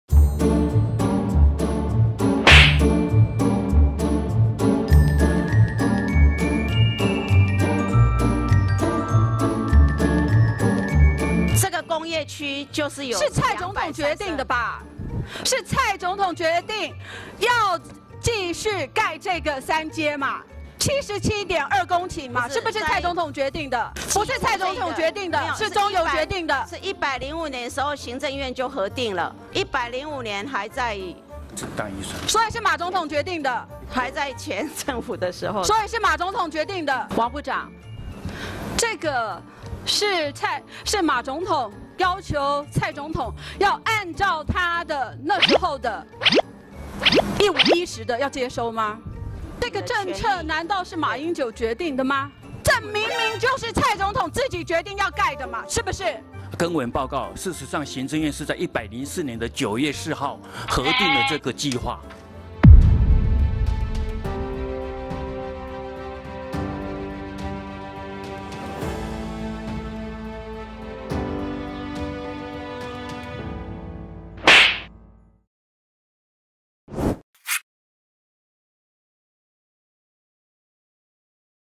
【國會零時差】國民黨立委高分貝質詢 慘遭經濟部光速打臉
國民黨立委謝衣鳳高分貝質詢，想把天然氣第三接收站的開發脈絡，胡亂移花接木，沒想到立刻被經濟部與中油光速打臉！

🎯天然氣第三接收站232公頃的方案，就是在馬政府時期核定
🎯蔡總統執政後，為了能源轉型、保護環境，不斷縮小、修正天然氣第三接收站的方案

請國民黨不要再為反而反，理性討論能源政策！